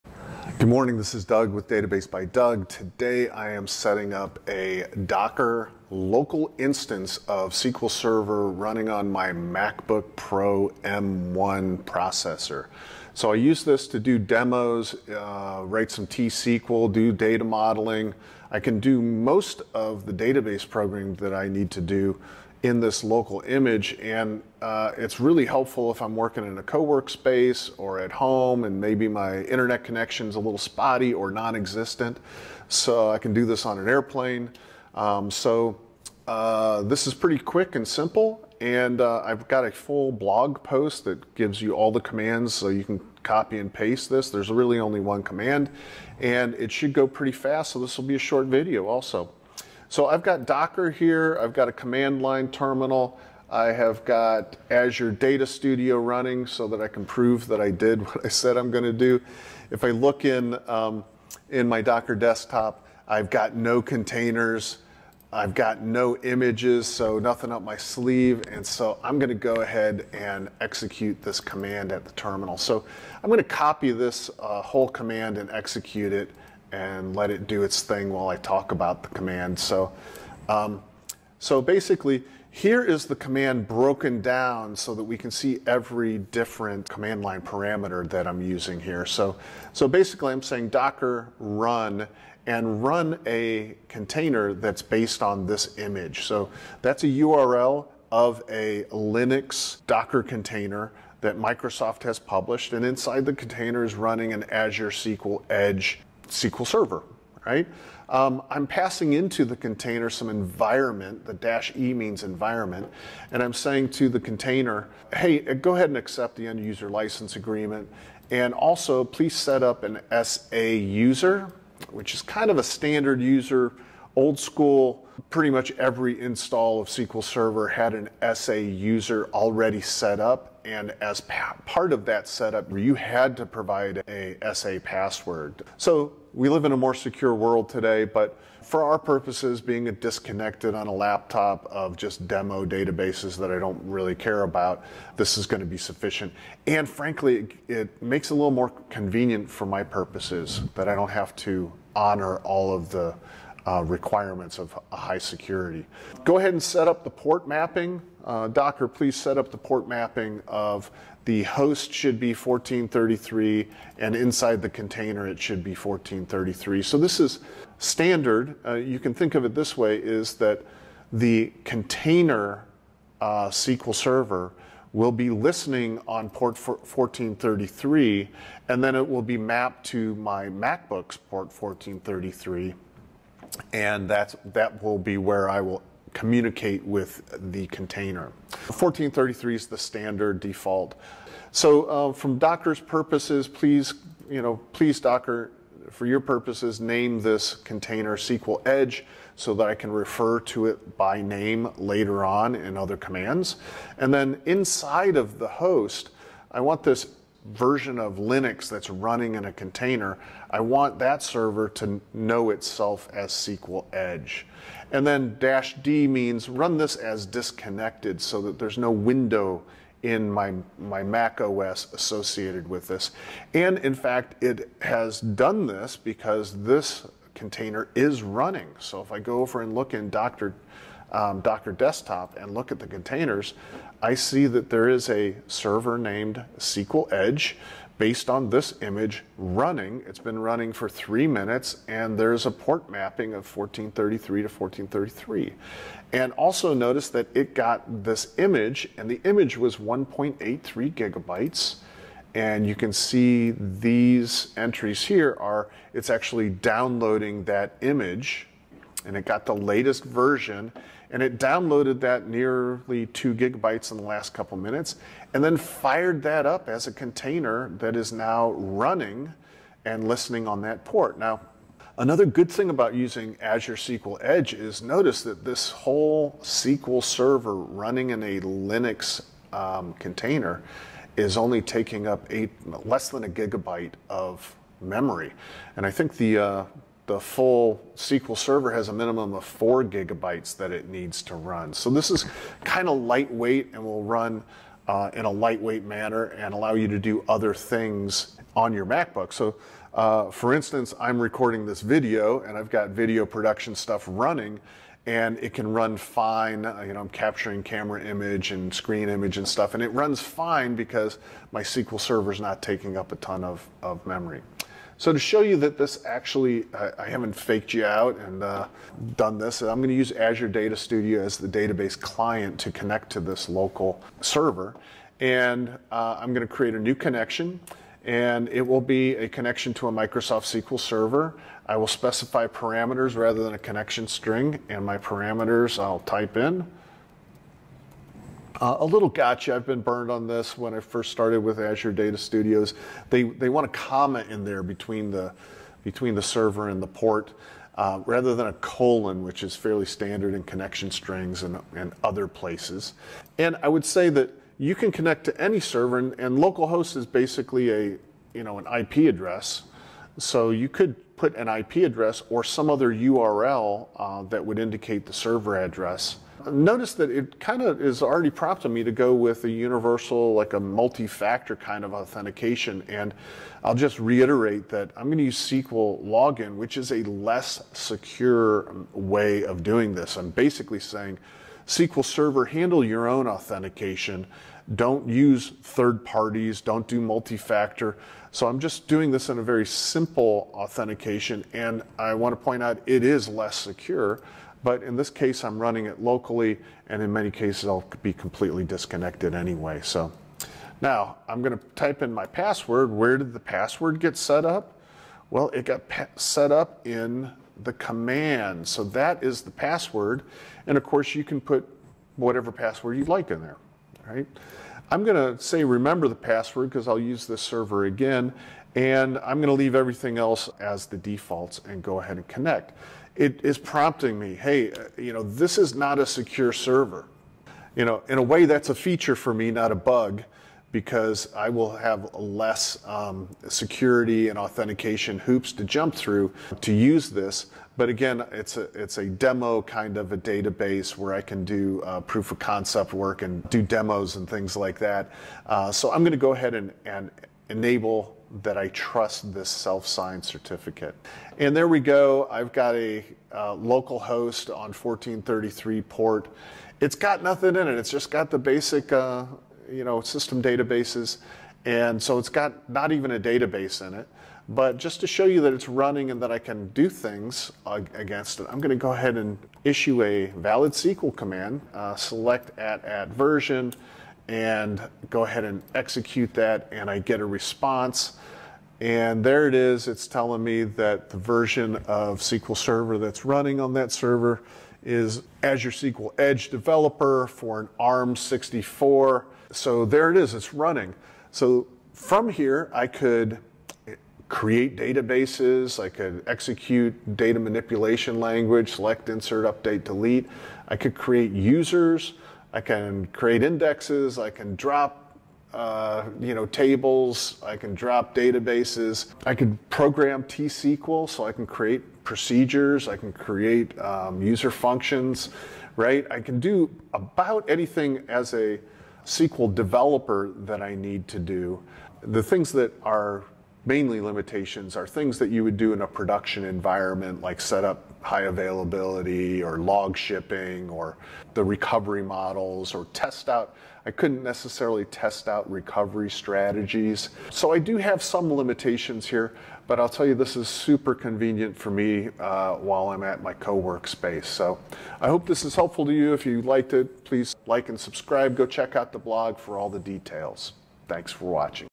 Run a SQL Server on your Macbook M1 2024
Setting up SQL Azure Edge as a Docker container on my Macbook Pro M1.

Great for SQL programming, data modeling, etc. Works without an internet connection. Lightweight instance runs in under 1GB memory.

See my blog post for the command and a full discussion of what each option does.